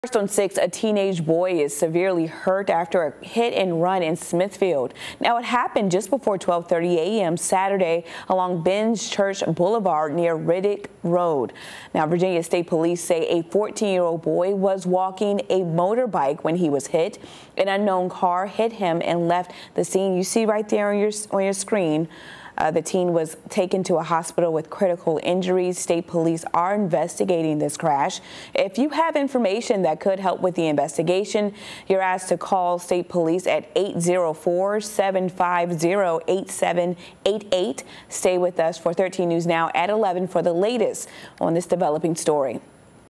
Boy, 14, seriously hurt in hit-and-run in Smithfield
Virginia State Police troopers responded to a report of a crash on Benn's Church Road not too far from the intersection with Riddick Road at around 12:25 a.m.